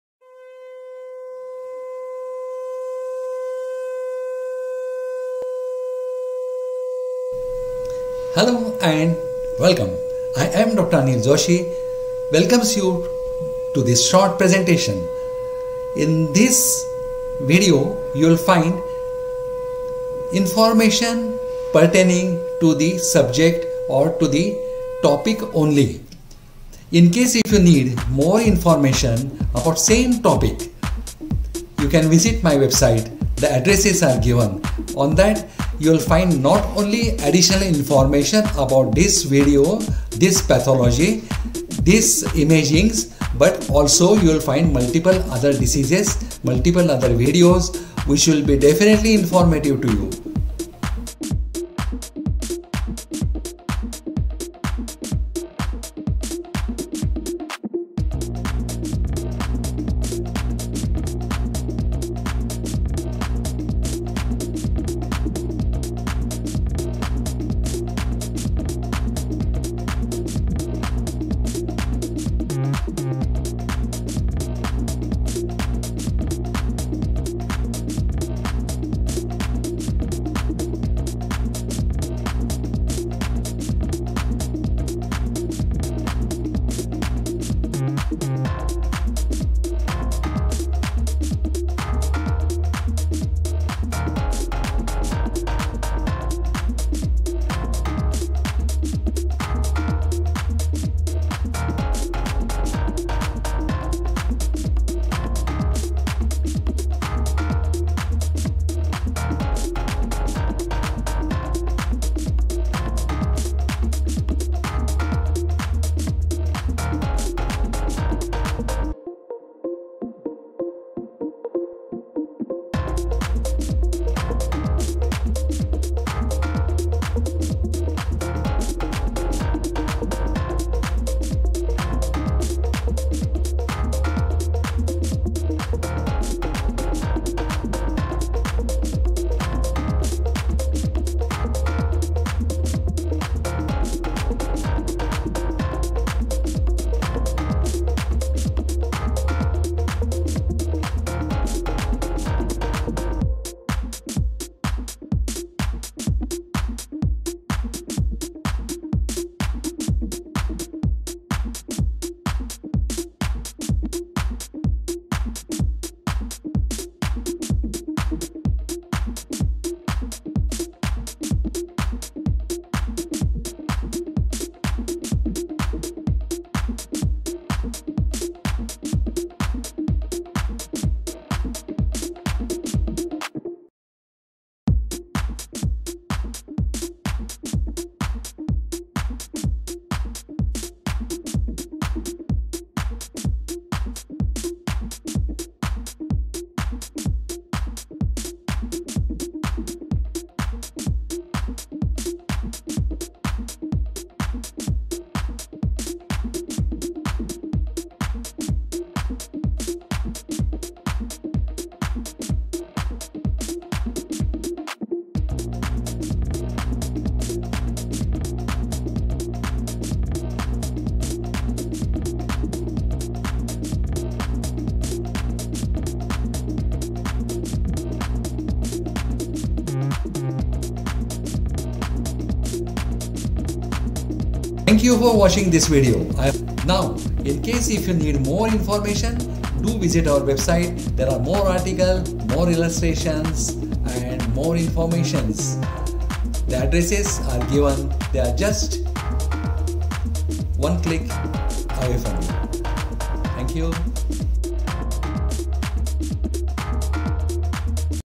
SCROTAL SONOGRAPHY PYOCELE - BY DR ANIL JOSHI.
SCROTAL SWELLING AND PAIN  IS A COMMON CLINICAL PROBLEM AND ALSO DIAGNOSTIC DILEMMA. THIS IMAGING GALLERY  ILLUSTRATES DIFFERENT IMAGING  PATTERNS IN DOPPLER SONOGRAPHY , BRIEFING DIFFERENTIAL DIAGNOSIS . THIS IS A CONDENSE VERSION OF THE EXTENSIVE LECTURE COVERED IN THE SERIES - LEARN RADIOLOGY WITH DR. ANIL JOSHI , AVAILABLE ON YOU TUBE AS WELL AS IN MY WEBSITE.